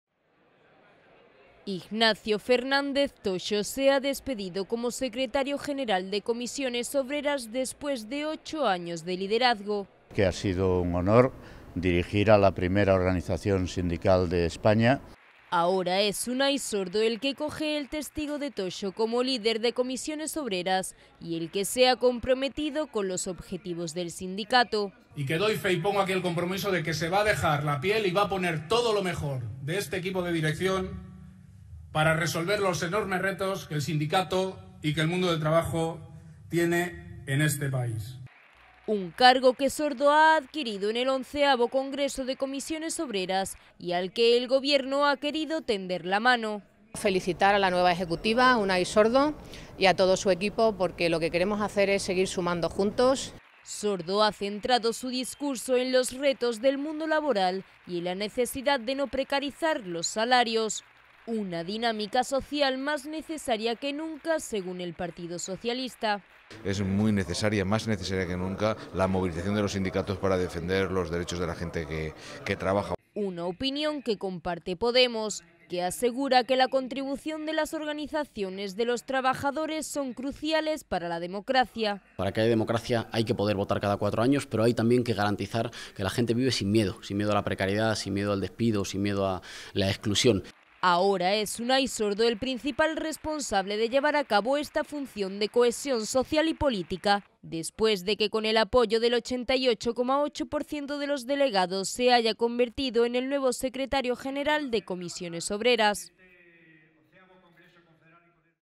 Unai Sordo, nuevo secretario general de CC.OO.
Ignacio Fernández Toxo se ha despedido como secretario general de Comisiones Obreras después de ocho años de liderazgo. Ahora es Unai Sordo el que coge el testigo de Toxo como líder de CC.OO. y el que se ha comprometido con los objetivos del sindicato.